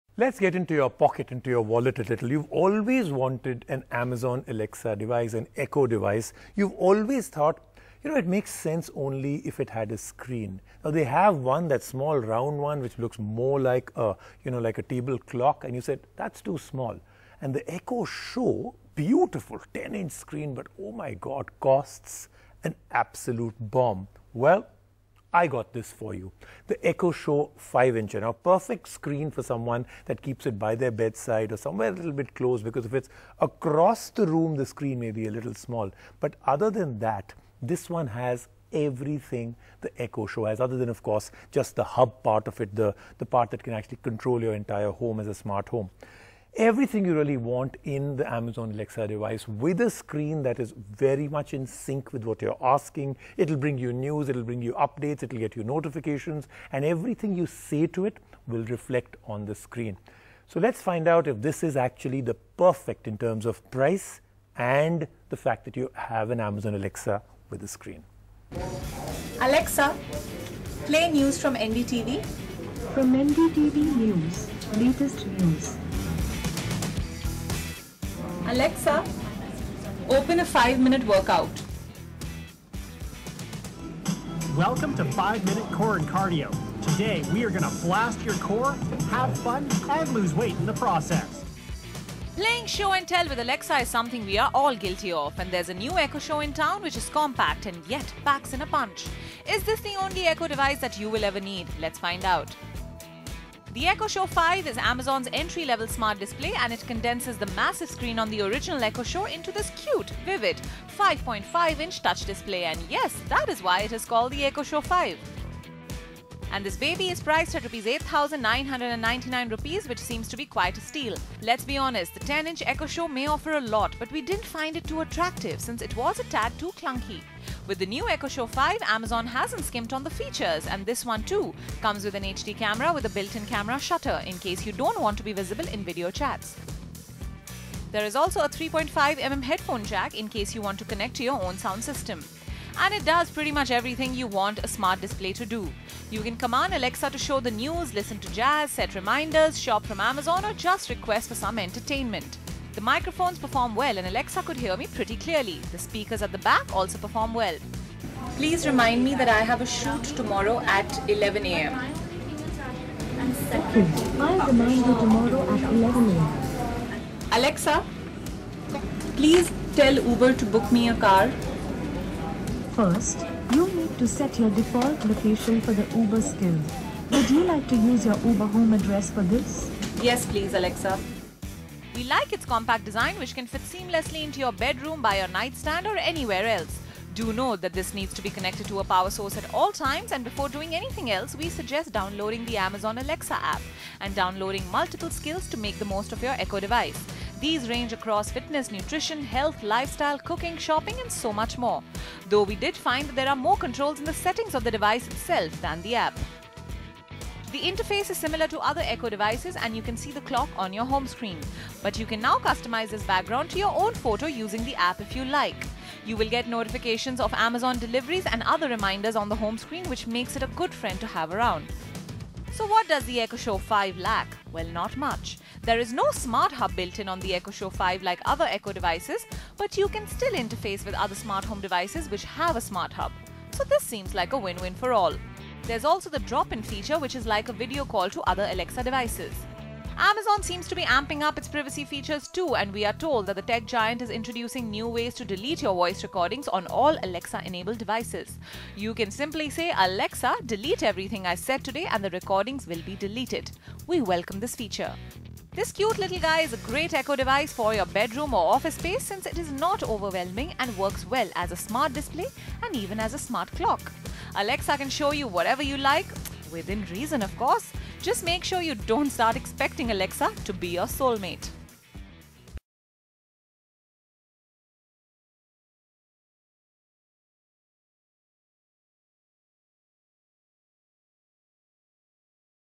Amazon Echo Show 5: The Only Echo Device You Need?
There's a more compact Echo device from Amazon and it boasts of all the features that you expect from the original Echo Show. We play show and tell with Alexa.